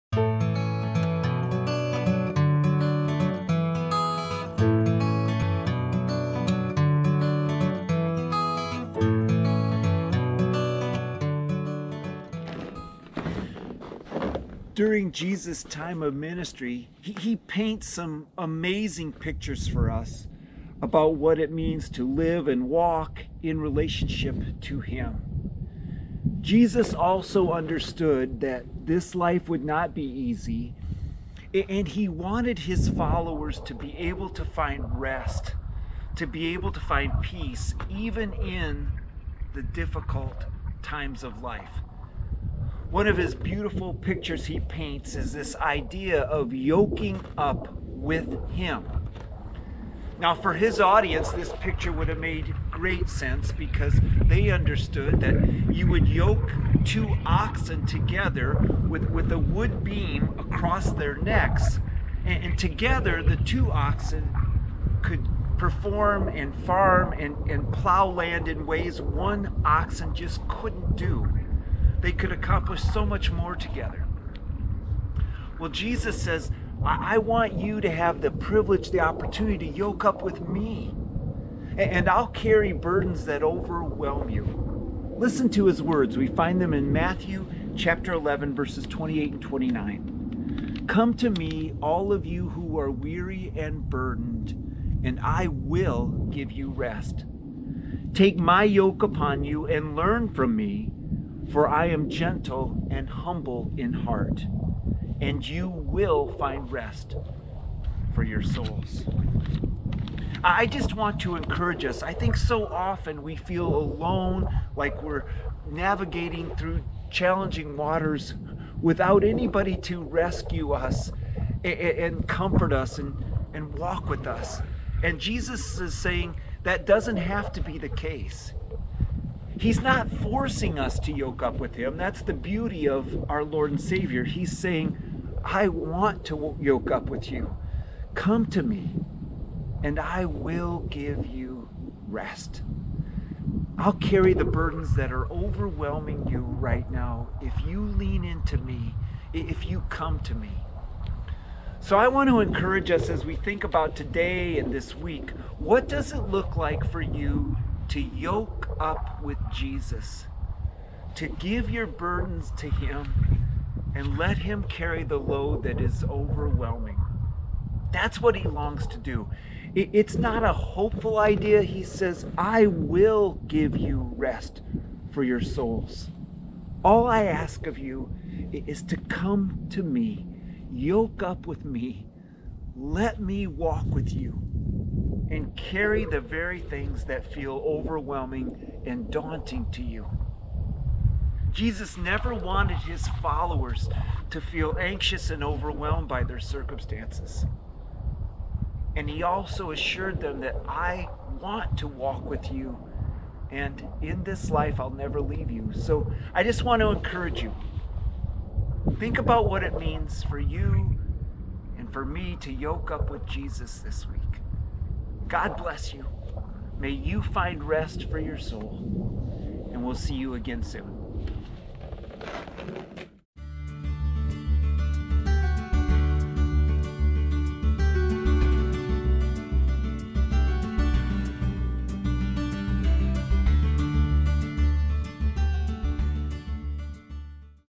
Soul Rest: Matthew 11:28-29
Thanks for joining us!  In today's devotion, we will be looking at the call of Jesus to come to Him when we are weary and burdened because He wants to give us rest!

Blessings, Pastor Chad